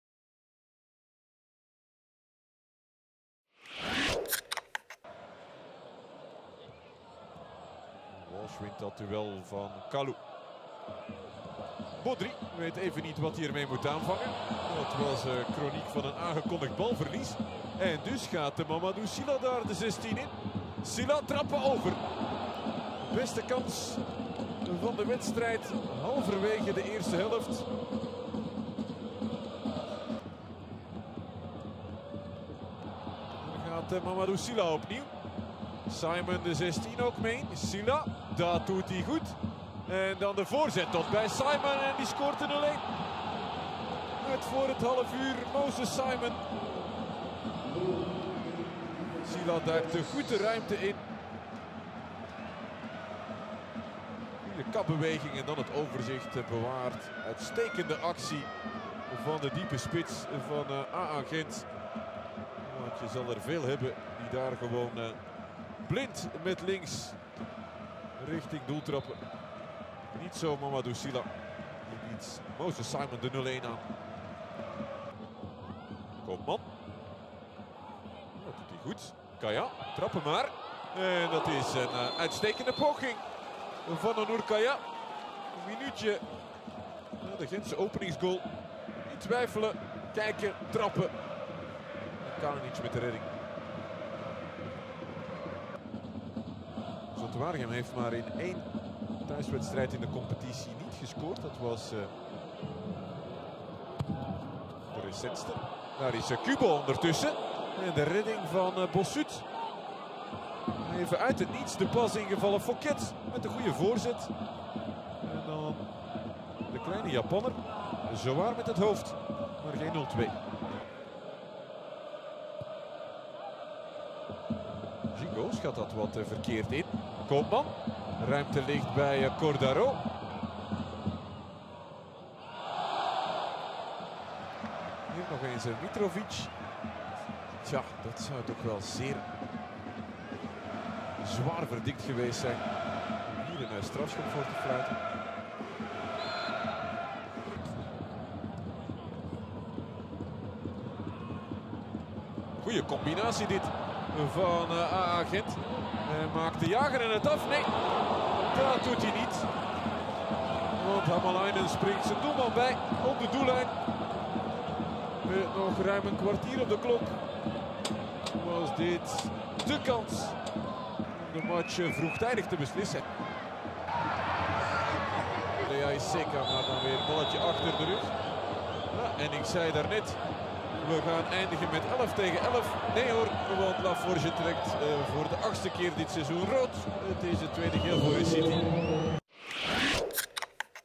🎬 Zulte-Waregem - KAA Gent (0-1)
Samenvatting van Zulte-Waregem - KAA Gent op speeldag 17 van de Jupiler Pro League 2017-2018.
FT: Zulte-Waregem - KAA Gent: 0-1
⚽ 0-1 Simon (28')